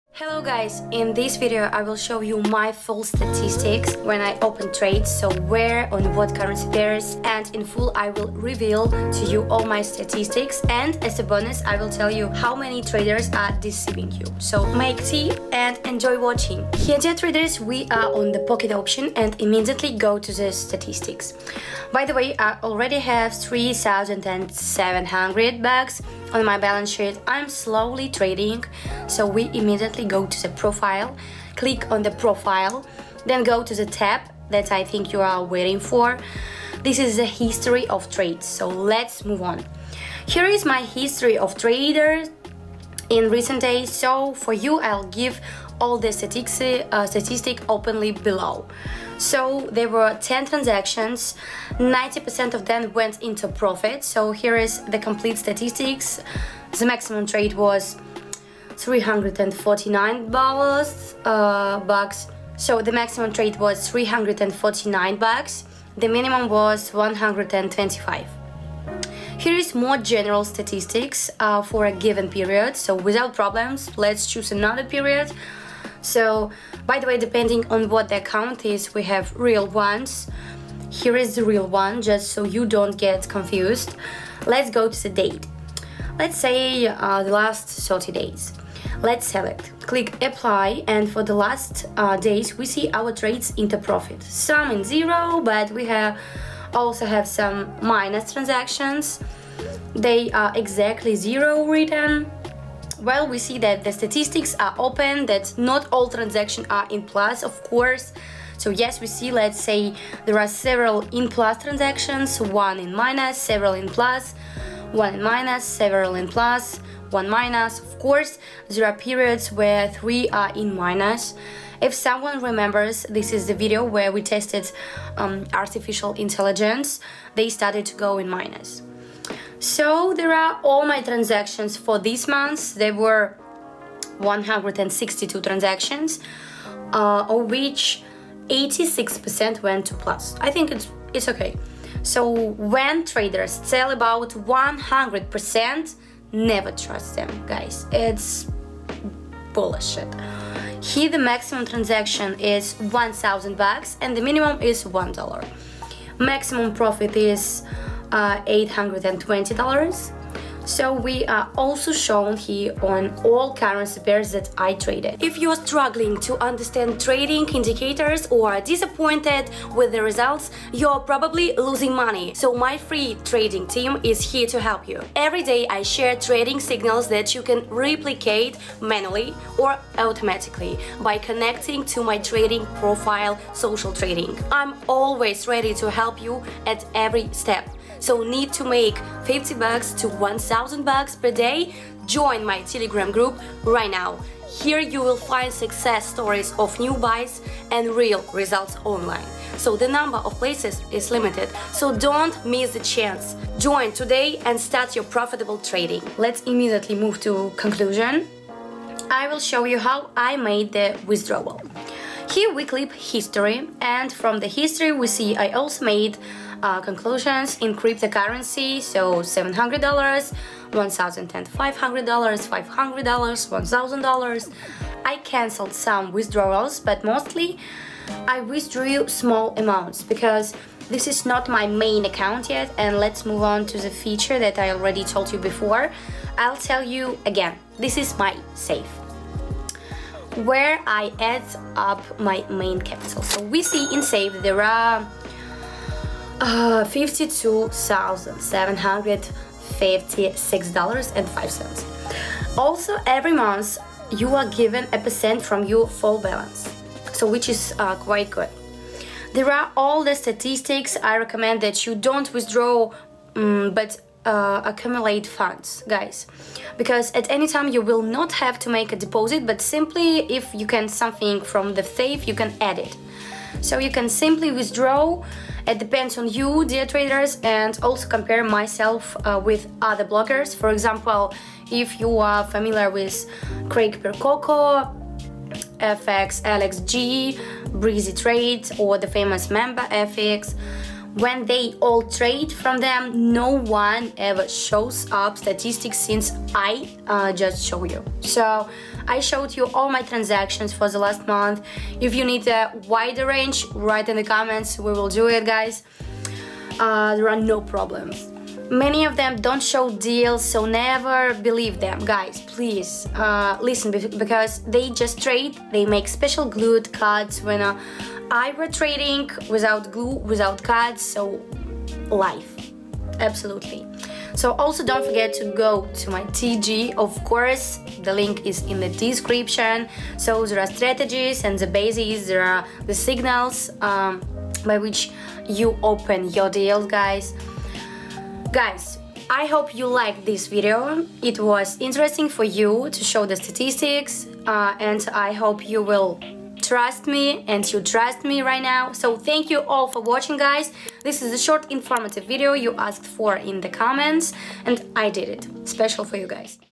📝 INSIDE MY POCKET OPTION PROFILE ➥ POCKET OPTION REVIEW 2024 ⭐ POCKET OPTION
✏️ Text me +TEAM

👉 Code: “JPU443”

⏱ Timestamps:
00:00 - Intro about my stats, how are you being scammed by other traders!!!?
00:18 - An overview of my stats on PO
03:36 - Withdrawal
04:09 - A review of my safe at the PO
04:49 - How do other traders scam you?

In this video, I'll show you how to create a successful binary options trading strategy using a stochastic oscillator. You'll see each trade I make and learn why I make those decisions. 😊📈

Binary options trading can be profitable if you understand technical tools and stock trading principles. My goal is to share this knowledge with you so you can make informed decisions and increase your chances of success. You'll find all the details in this video! 📹💰